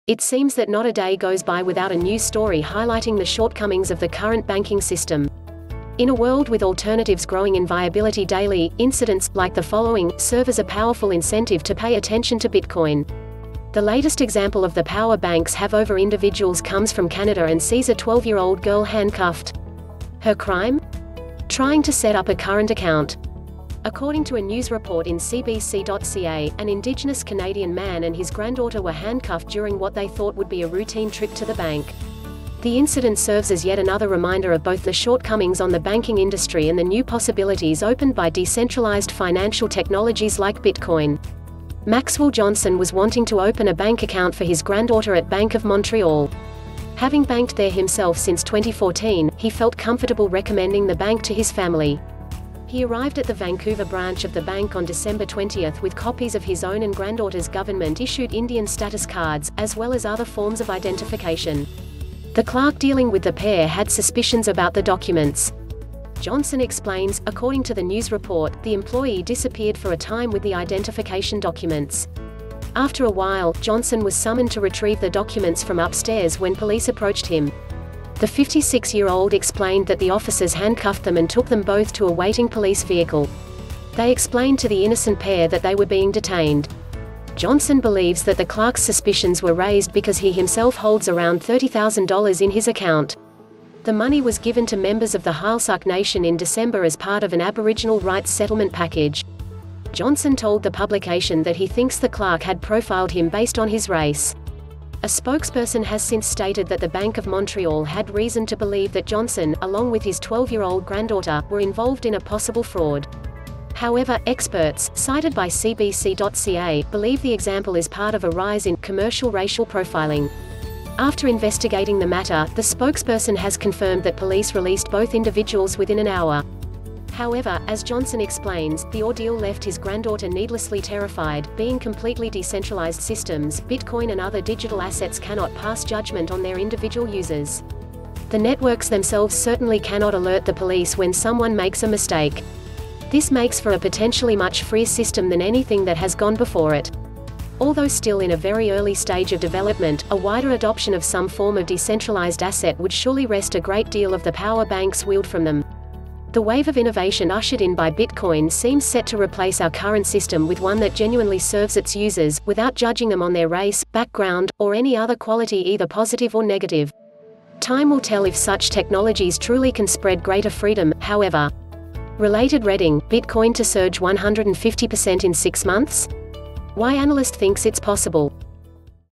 Bitcoin Can’t Racially Profile You, Unlike Your Banking Provider
It seems that not a day goes by without a news story highlighting the shortcomings of the current banking system. In a world with alternatives growing in viability daily, incidents, like the following, serve as a powerful incentive to pay attention to Bitcoin. The latest example of the power banks have over individuals comes from Canada and sees a 12 year old girl handcuffed. Her crime? Trying to set up a current account. According to a news report in CBC.ca, an indigenous Canadian man and his granddaughter were handcuffed during what they thought would be a routine trip to the bank. The incident serves as yet another reminder of both the shortcomings on the banking industry and the new possibilities opened by decentralised financial technologies like Bitcoin. Maxwell Johnson was wanting to open a bank account for his granddaughter at Bank of Montreal. Having banked there himself since 2014, he felt comfortable recommending the bank to his family. He arrived at the Vancouver branch of the bank on December 20 with copies of his own and granddaughter’s government-issued Indian Status cards, as well as other forms of identification. The clerk dealing with the pair had suspicions about the documents. Johnson explains:According to the news report, the employee disappeared for a time with the identification documents. After a while, Johnson was summoned to retrieve the documents from upstairs when police approached him. The 56 year old explained that the officers handcuffed them and took them both to a waiting police vehicle. They explained to the innocent pair that they were being detained. Johnson believes that the clerk’s suspicions were raised because he himself holds around $30,000 in his account. The money was given to members of the Heiltsuk nation in December as part of an Aboriginal rights settlement package. Johnson told the publication that he thinks the clerk had profiled him based on his race. A spokesperson has since stated that the Bank of Montreal had reason to believe that Johnson, along with his 12 year old granddaughter, were involved in a “possible fraud”. However, experts, cited by CBC.ca, believe the example is part of a rise in “commercial racial profiling”. After investigating the matter, the spokesperson has confirmed that police released both individuals within an hour. However, as Johnson explains, the ordeal left his granddaughter needlessly terrified:Being completely decentralised systems, Bitcoin and other digital assets cannot pass judgement on their individual users. The networks themselves certainly cannot alert the police when someone makes a mistake. This makes for a potentially much freer system than anything that has gone before it. Although still in a very early stage of development, a wider adoption of some form of decentralised asset would surely wrest a great deal of the power banks wield from them. The wave of innovation ushered in by Bitcoin seems set to replace our current system with one that genuinely serves its users, without judging them on their race, background, or any other quality either positive or negative. Time will tell if such technologies truly can spread greater freedom, however. Related Reading: Bitcoin to Surge 150% In 6 Months? Why Analyst Thinks It’s Possible